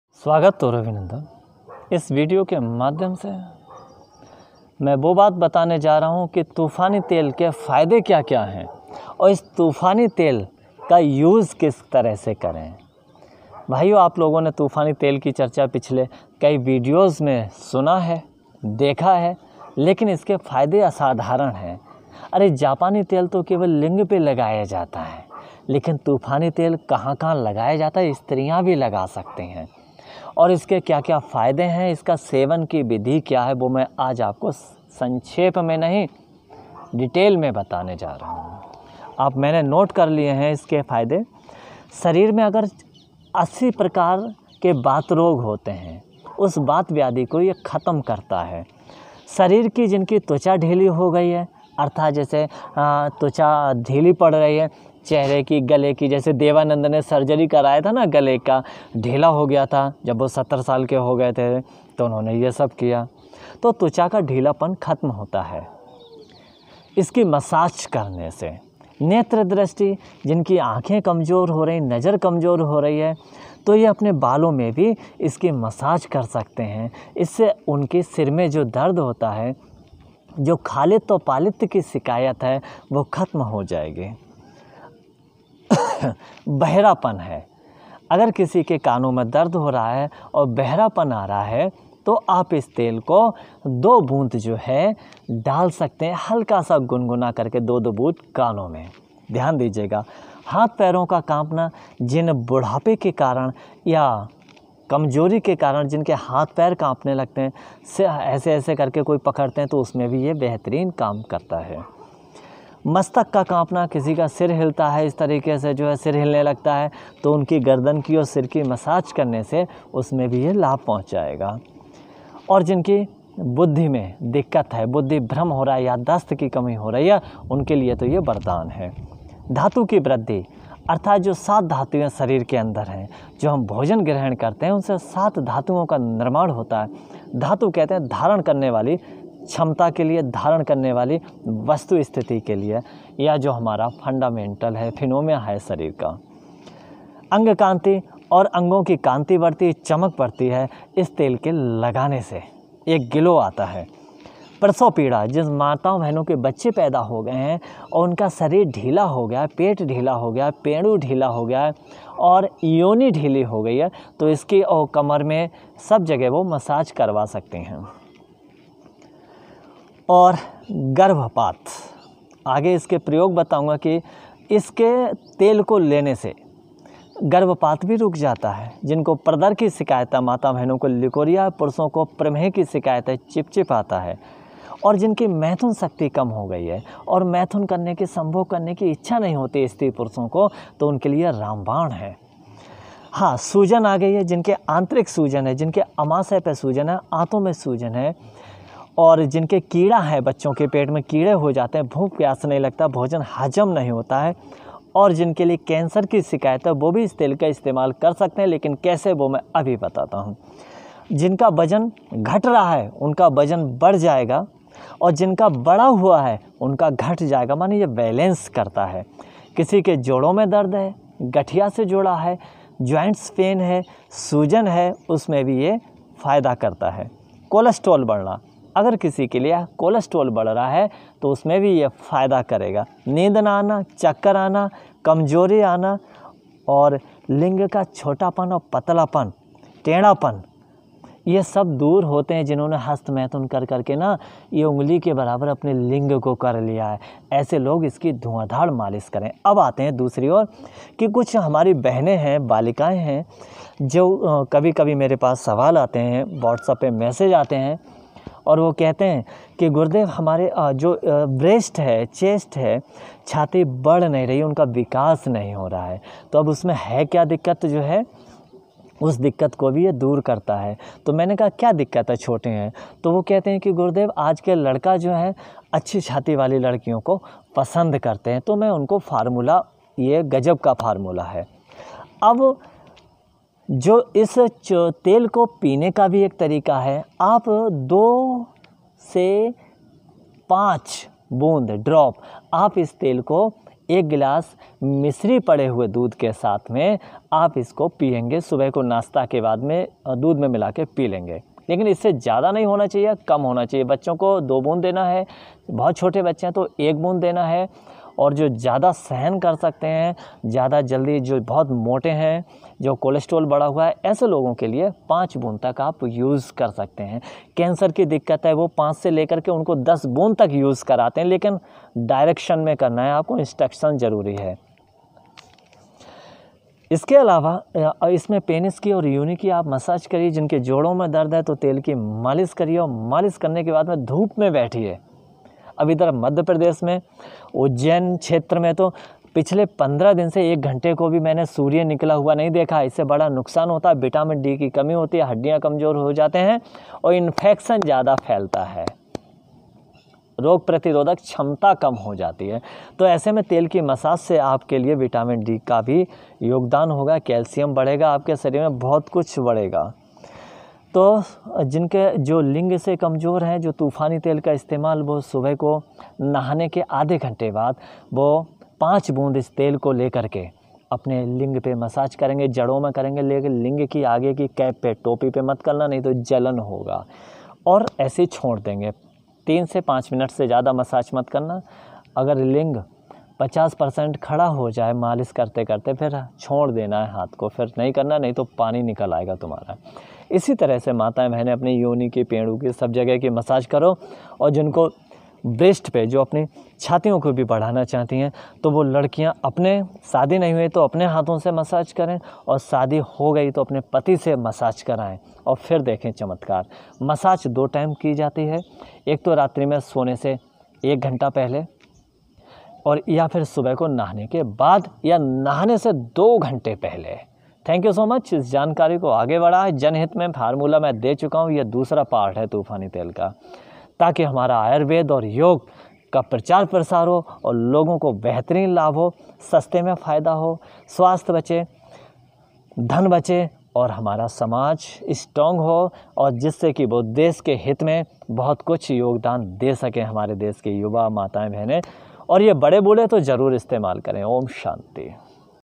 तूफानी-तेल👈के 50 फायदे व (लिंग-योनि)की मालिश💕विधि के साथ / तूफानी तिला पार्ट-2 English subtitle
धन्यवाद मित्रों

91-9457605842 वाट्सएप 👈